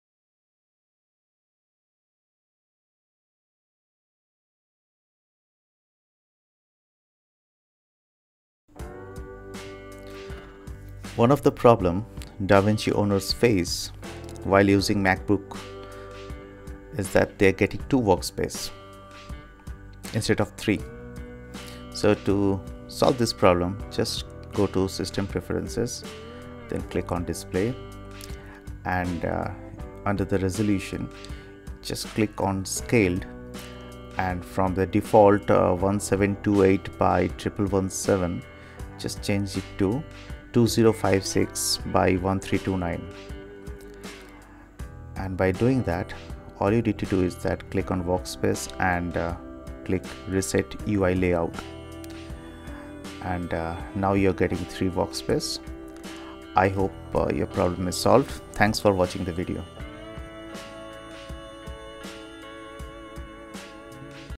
DAVINCI RESOLVE - Only two workspaces in color tab instead of three - Macbook
solving the problem of getting two workspaces in davinci resolve instead of three.